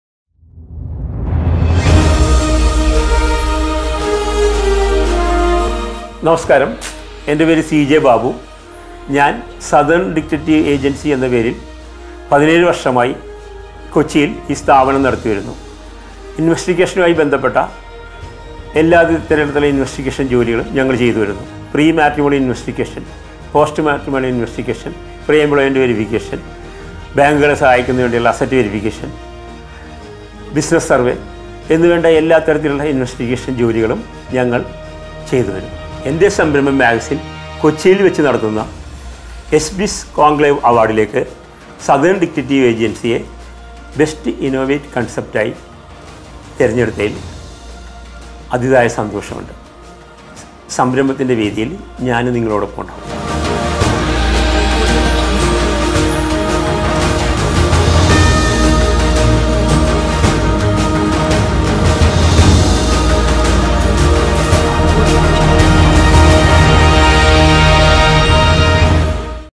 BEST INNOVATIVE BUSINESS CONCEPT/SOUTHERN DETECTIVE AGENCY/YES BIZ CONCLAVE & AWARDS 2020
YES BIZ CONCLAVE & AWARDS 2020
In the world of entrepreneurship, there are many young entrepreneurs from our own Kerala who remain as the perfect example for all those who dream of entrepreneurship. We have got many things to learn from them including the ways to increase sales, how to find the best employees suited to their business and how they grow despite of facing all hardships. YES BIZ CONCLAVE AND AWARDS 2020 is the best venue for all those who want to know all these things and learn from them. The struggling entrepreneurs who can't find or set a goal, those who want to know more about the legal formalities associated with each business and thereby to increase sales... whoever it be or whatever your problems associated with your business, make the best use of YES BIZ CONCLAVE AND AWARDS 2020.
VENUE  : HOLIDAY INN
PLACE  : Kochi
DATE : 22nd MARCH